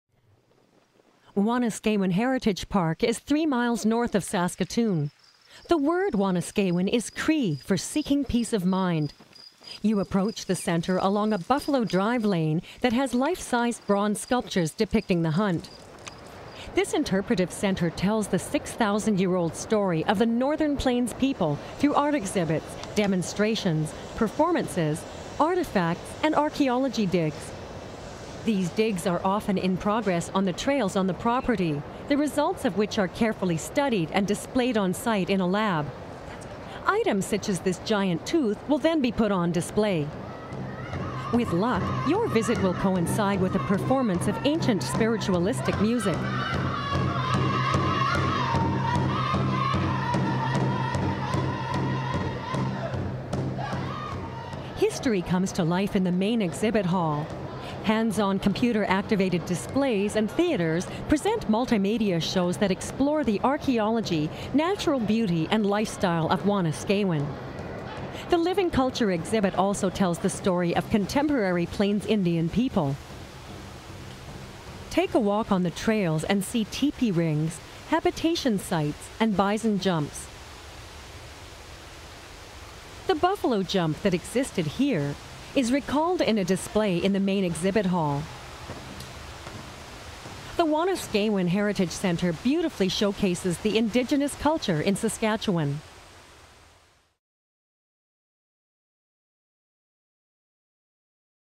Wanuskewin Heritage Park of Saskatchewan
Wanuskewin Heritage Park is three miles north of Saskatoon. The word, Wanuskewin, is Cree for seeking peace of mind. Within the park is an interpretive center that tells the 6,000-year-old story of the Northern Plains people, by beautifully showcasing the indigenous culture in Saskatchewan. Wanuskewin also contains 19 pre-contact sites, all clustered within half a mile of each other. The Northern Plains Indians gathered here for thousands of years, hunting bison and gathering food to escape the winter winds.